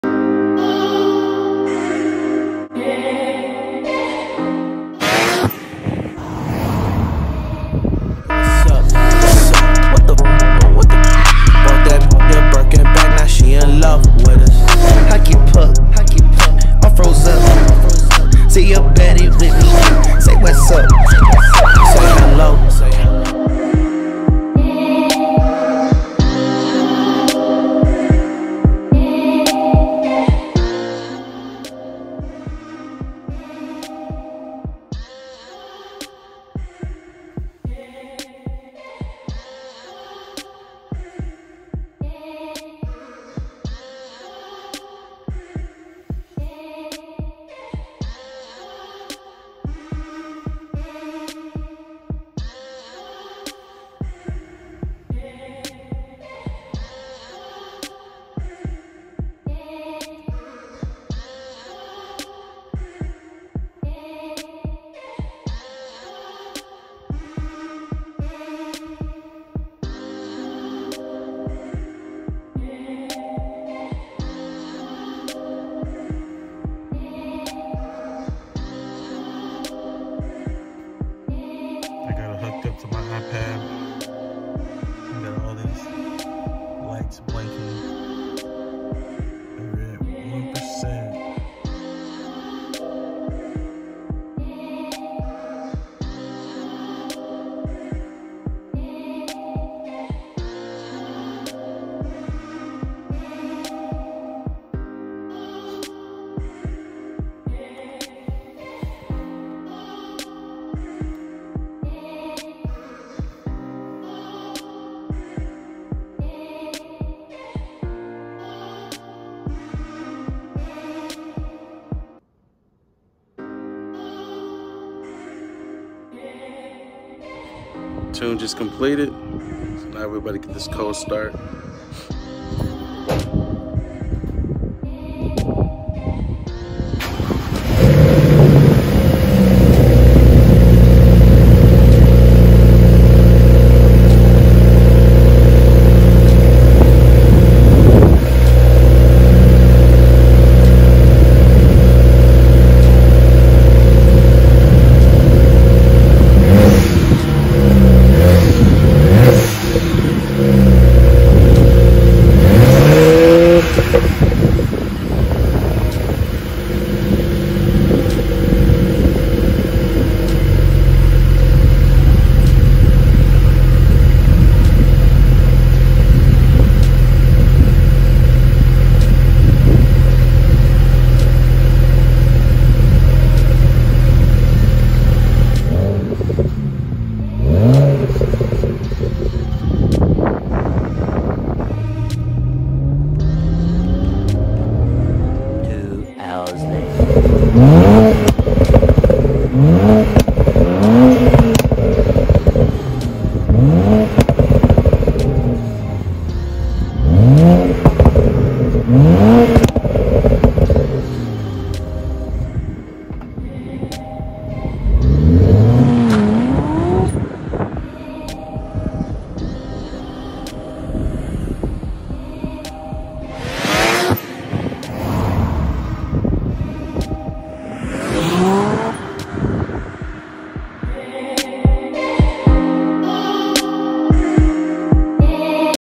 MHD Tuning on BMW 335i
On the Road to 500 Horse Power Installing MHD Tuning on my BMW 335i w/ Dual intake cone and Burger Charge Pipe &BOV and Upgraded fuel injectors and Upgrades Coil Packs and Spark Plugs